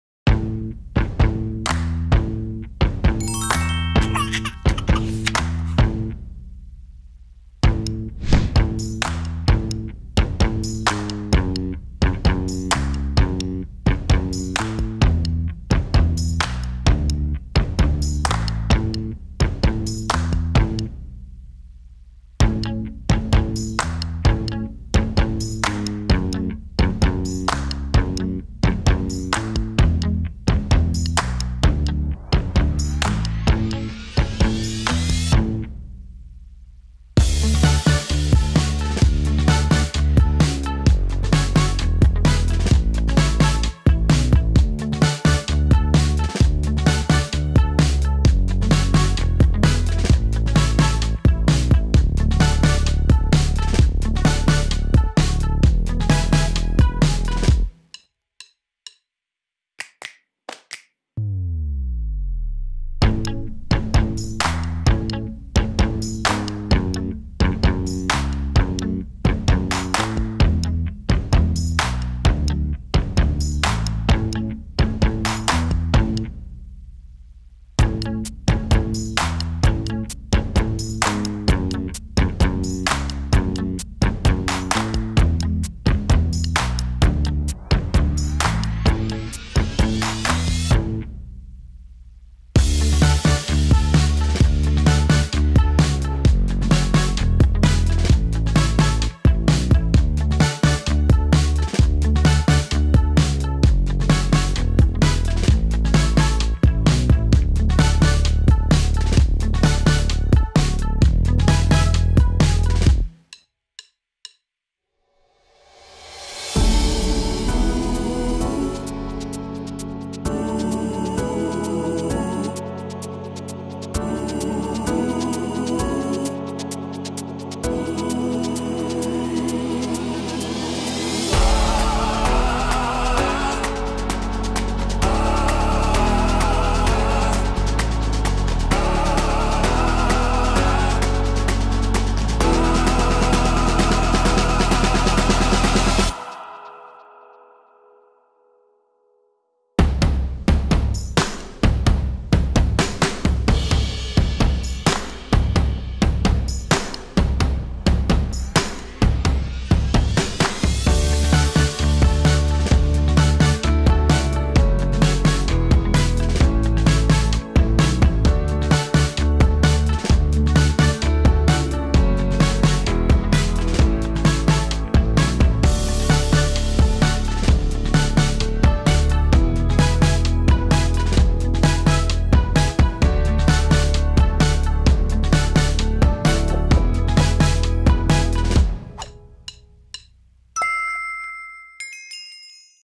Easy PAPER CRAFT fold heart note | Kids Crafts DIY Origami اوريغامي 折り紙 оригами 종이접기
Learn how to make a simple and easy fold heart note origami step by step.
How to Make a fold heart note origami
Here's Kids Crafts to make Origami fold heart note with the kids. Just a couple sheets of construction paper and glue is all you will need.